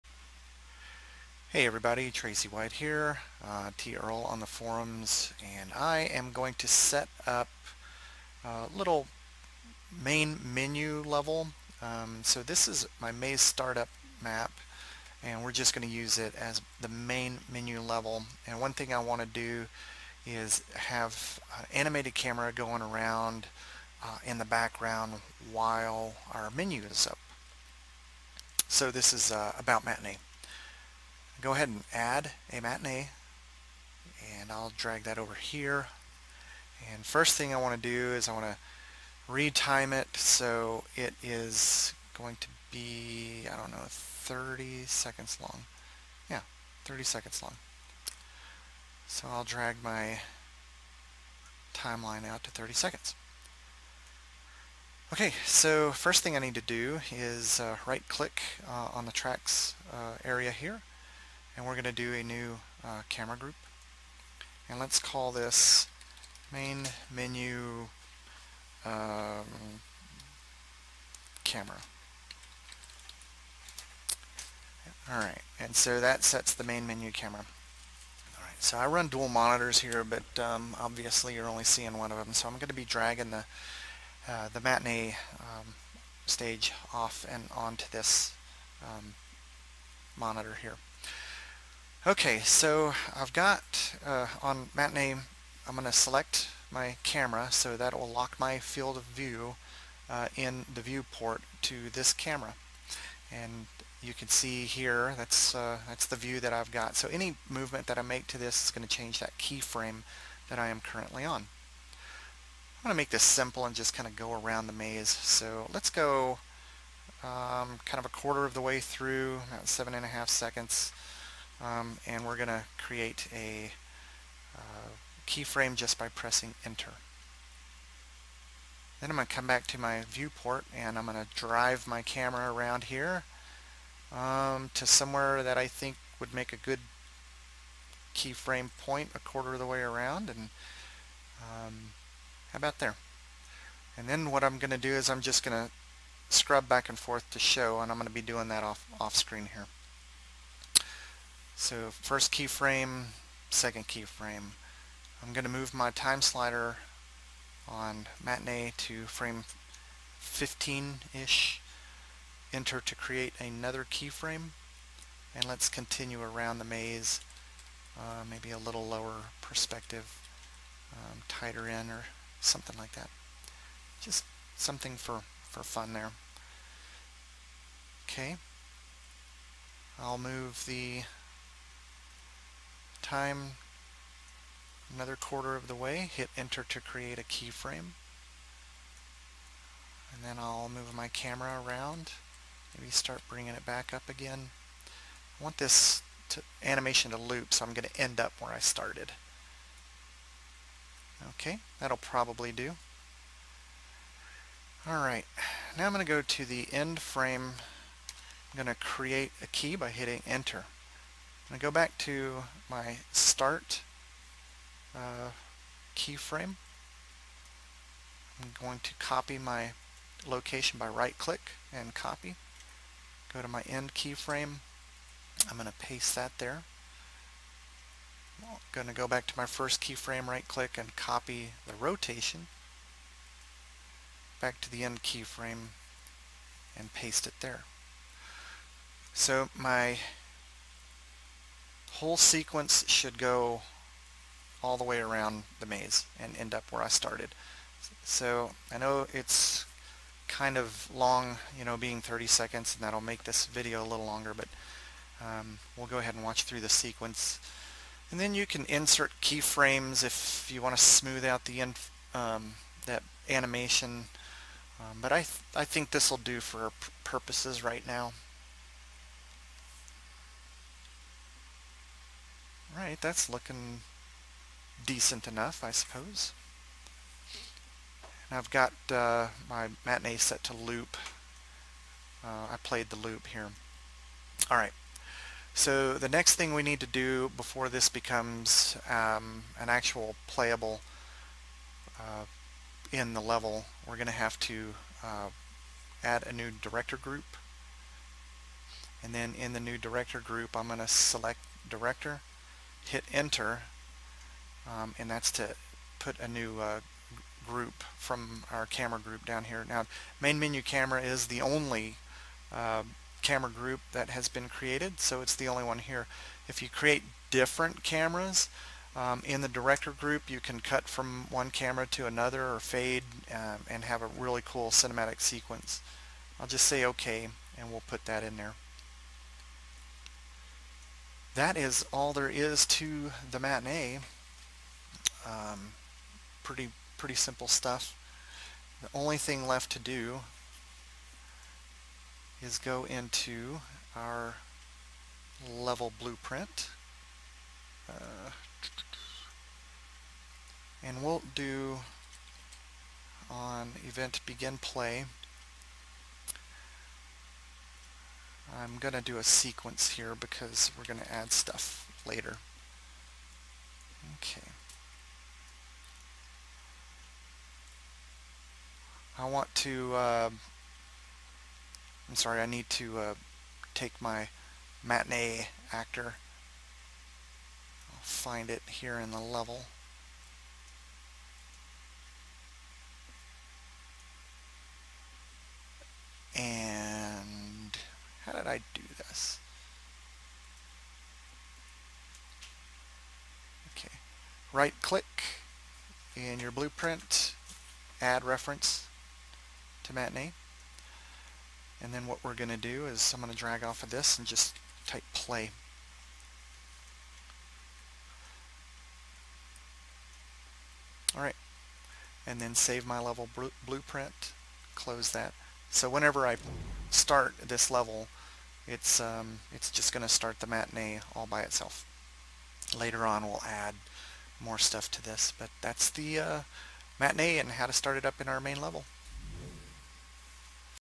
8 - UE4: Setting up a simple Matinee sequence
This is the eighth in my video series on creating a game in UE4. This follows my process in making "a-Maze", my simple game using the Unreal Engine 4.
In this video, I create a very simple Matinee sequence and make it start up through the level blueprint when the level load
Please leave questions and comments in the comments section.
Thanks!